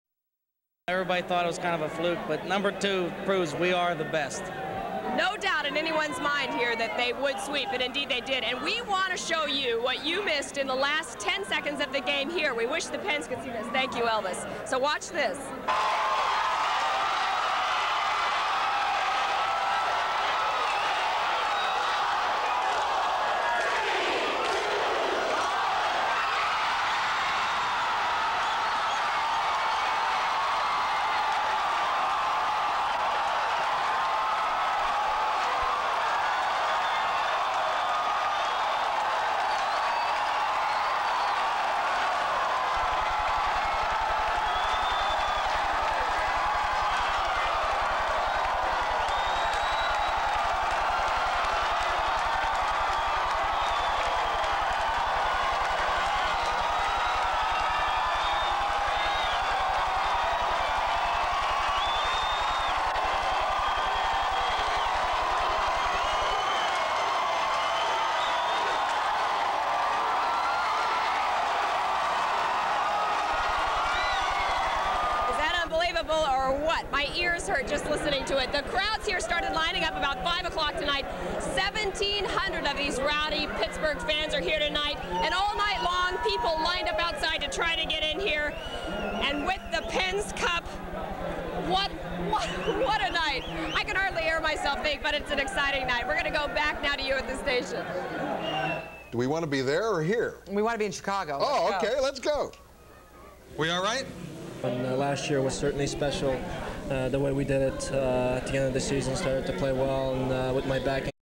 1992 Pens* Elvis has Just Left the Building WTAE - Sheila Hyland
The wild celebration after the Pens win their second cup